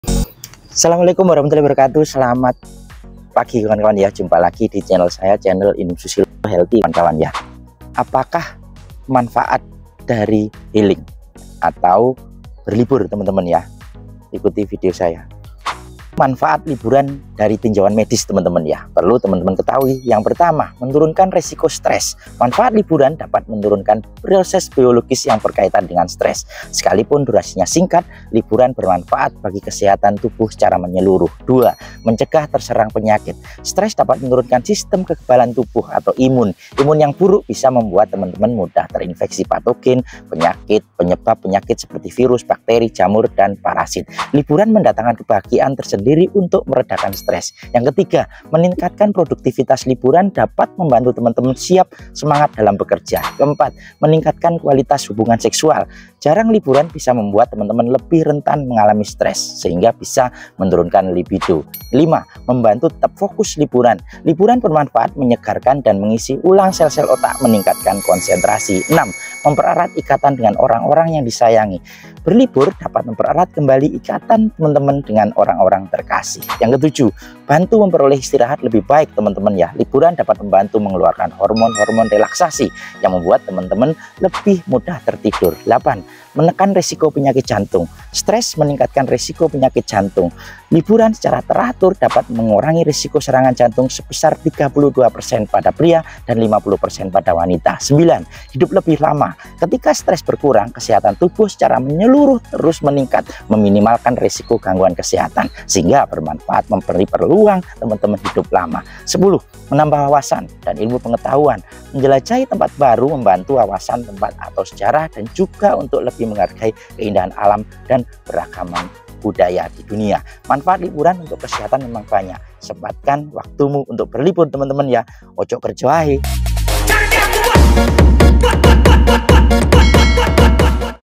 10 MANFAAT SEHAT LiBURAN atau HEALING YANG JARANG DiKETAHUi
Kawan nawak disini Saya Share Info Kesehatan 10 MANFAAT SEHAT HEALiNG LiBURAN YANG JARANG DiKETAHUi, Semoga Bermanfaat

Pesan
Jaket Rompi Hoodi Custom
suka suka

Tonton lengkap juga
Info sehat Lengkap